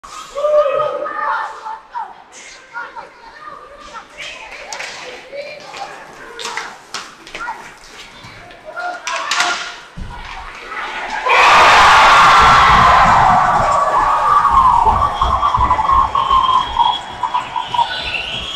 Clara Van Wieren (USA) goal - 2020 IIHF U18 Women's World Championship
USA v.s Finland - Dec 26th 2019